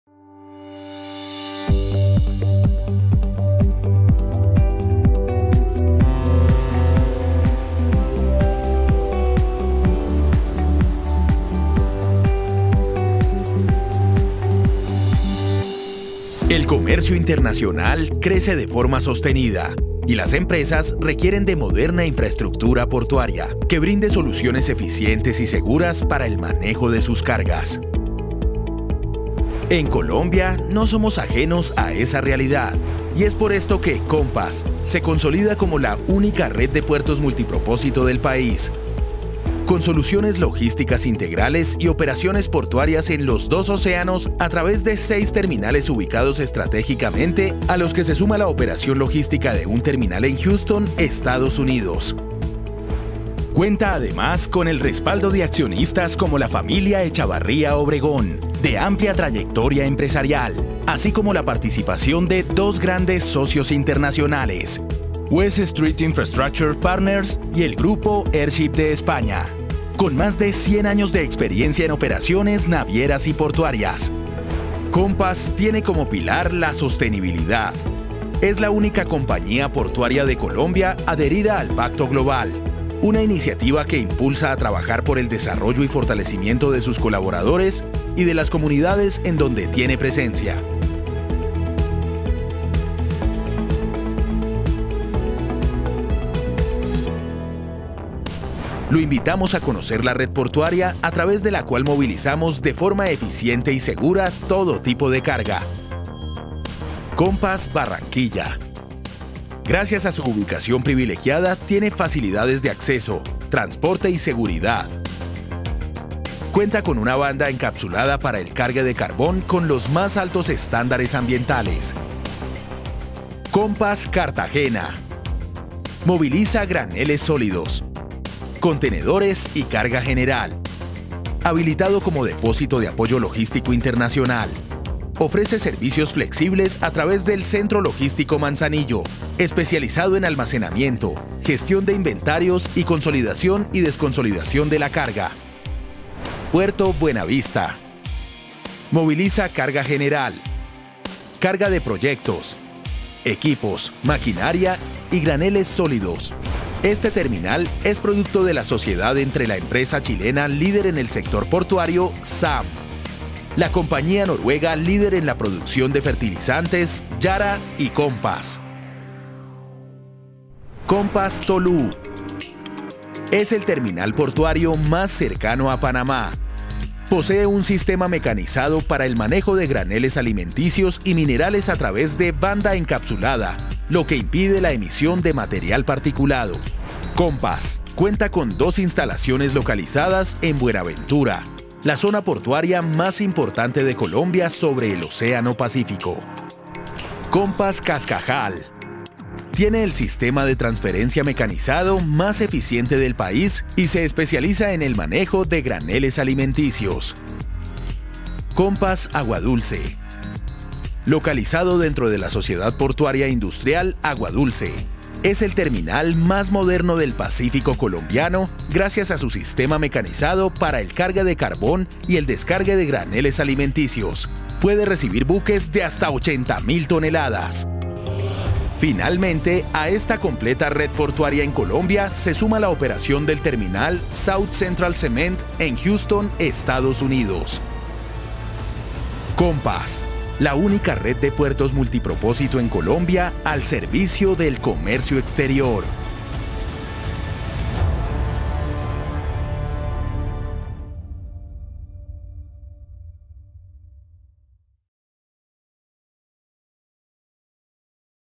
Video corporativo Compañía de Puertos Asociados - Compas
Compas es la única red de puertos multipropósito de Colombia con seis terminales portuarios en operación localizados en Cartagena, Barranquilla, Tolú y Buenaventura, a estos se suma una operación internacional en Houston, Estados Unidos.